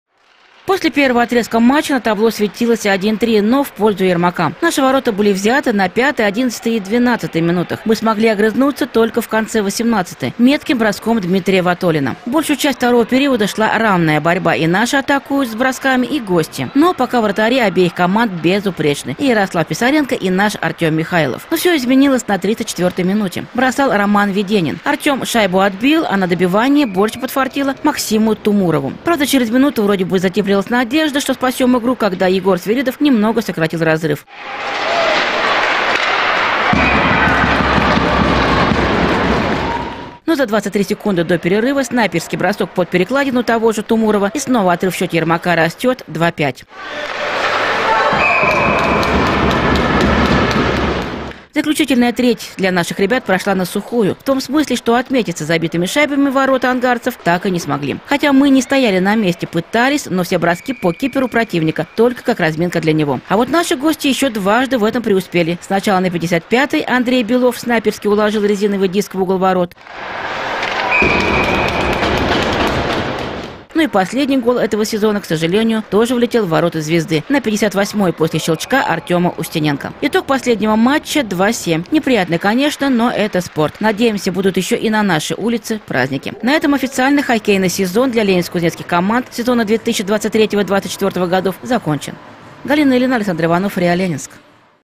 Новости спорта
В «Ледовом дворце спорта» Ленинска-Кузнецкого, 16 и 17 апреля прошли два заключительных матча Первенства Сибирского федерального округа по хоккею с шайбой, среди юношей до 14 лет. Ленинск-Кузнецкая «Звезда» принимала команду с берегов Ангары – «Ермак». Итоги первой встречи вы знаете из вчерашнего нашего сюжета, а сегодня про игру 17 апреля.